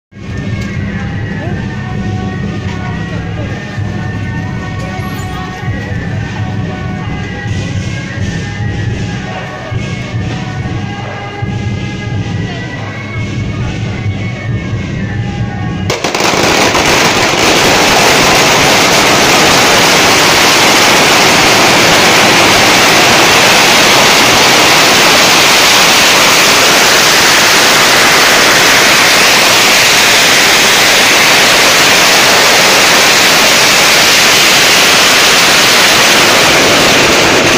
【台北元宵活動】內湖梘頭福德祠-夜弄土地公求平安龜
台北內湖人才知
梘頭福德祠-夜弄土地公
元宵限定活動開始了
越夜越熱鬧